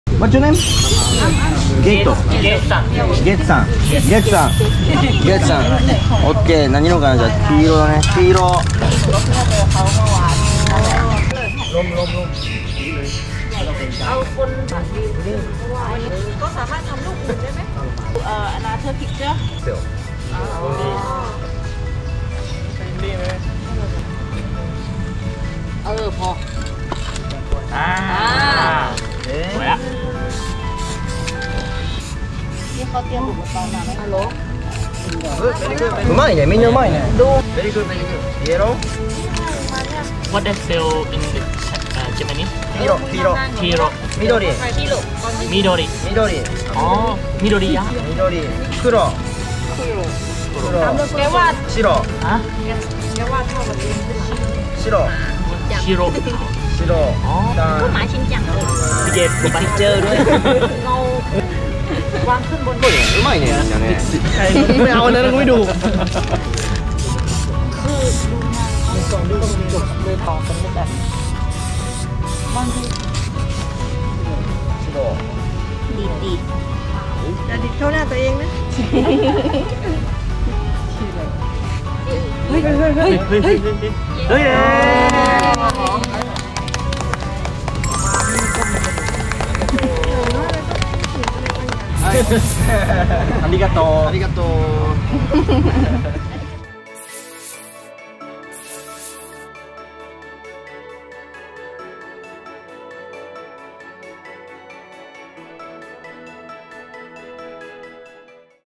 プロが海外のJDにスプレーアート教えてみたらめちゃくちゃ上手かった！！
※この動画は去年撮影したものです